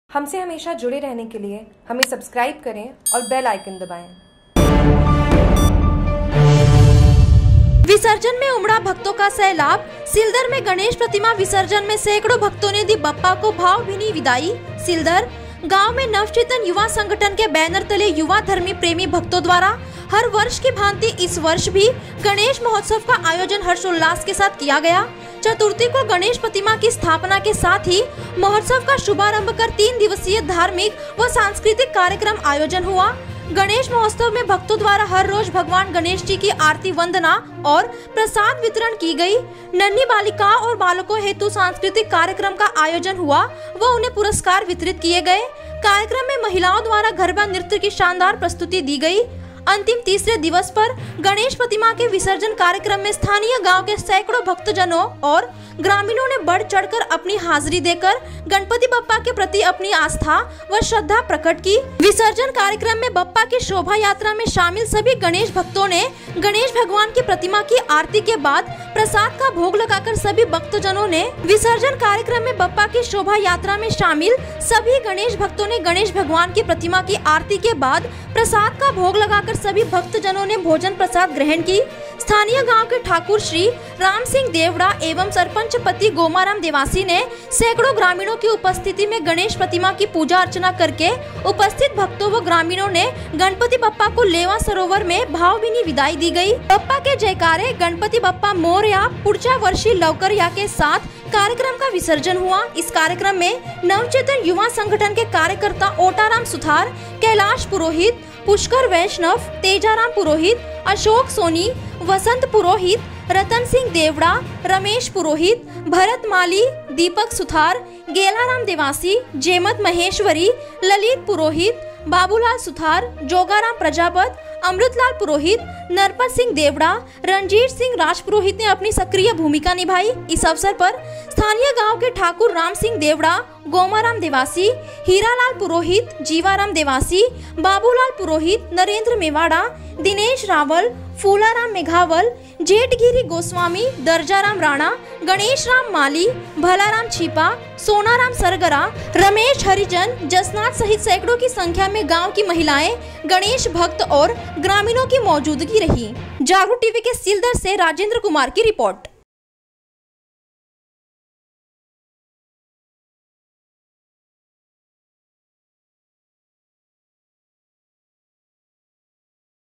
सिलदर : विसर्जन में उमड़ा भक्तों का सैलाब
गांव में नवचेतन युवा संगठन के बैनर तले  युवा धर्मी प्रेमी भक्तों द्वारा हर वर्ष की भांति इस वर्ष भी  गणेश महोत्सव का आयोजन हर्षोल्लाष के साथ किया गया।
चतुर्थी को गणेश प्रतिमा की स्थापना के साथ ही महोत्सव का शुभारंभ कर तीन दिवसीय धार्मिक व सांस्कृतिक कार्यक्रम का आयोजन हुआ।
गणेश महोत्सव में भक्तों द्वारा हर रोज भगवान गणेशजी की आरती-वन्दना और प्रसाद वितरण की गई। नन्ही बालिकाओं और बालकों हेतु सांस्कृतिक कार्यक्रम का आयोजन हुआ व उन्हें पुरुष्कार वितरित किये गये। कार्यक्रम में महिलाओं द्वारा गरबा नृत्य की शानदर प्रस्तुति दी गई।
अंतिम तीसरे दिवस पर गणेश प्रतिमा के विसर्जन कार्यक्रम में स्थानीय गाँव के सैकड़ो भक्तजनों और ग्रामीणों ने  बढ़चढ़ कर अपनी हाजरी देकर गणपति बप्पा के प्रति अपनी आस्था व श्रद्धा प्रकट की।
विसर्जन कार्यक्रम में बप्पा की शोभायात्रा में शामिल सभी गणेश भक्तों ने गणेश भगवान की प्रतिमा की आरती के बाद प्रसाद का भोग लगा कर सभी भक्तजनों ने भोजन प्रसाद ग्रहण की।
स्थानीय गांव के ठाकुर श्री रामसिंह देवड़ा एवं सरपंच पति गोमाराम देवासी ने सैकड़ो ग्रामीणों की उपस्तिथि में गणेश प्रतिमा की पूजा-अर्चना करके उपस्तिथ  भक्तों व ग्रामीणों ने गणपति बप्पा को लेवा सरोवर में भावभीनी विदाई दी गई।
बप्पा के जयकारे...गणपति बप्पा मोरया...पुढच्या वर्षी लवकर या के साथ कार्यक्रम का विसर्जन हुआ